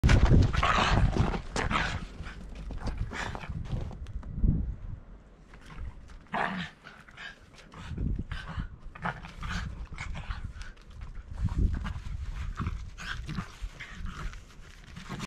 巻き込まれそうになって怖かったと訴える3児の母
🥓こんにちわマヨネといいます。毎朝6時に裏庭で自由に暮らしている3匹の柴犬親子との交流を投稿しております。